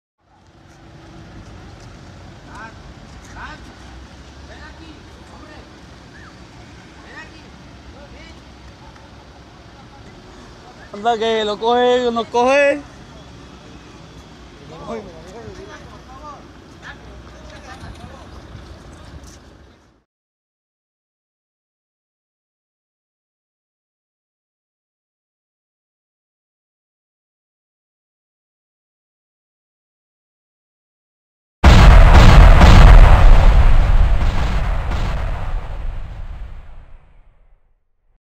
Escaped circus hippo makes a desperate bid for freedom across busy road in jaw-dropping footage
In the amazing clip the huge beast can be seen wandering across a busy carriageway as frightened pedestrians back away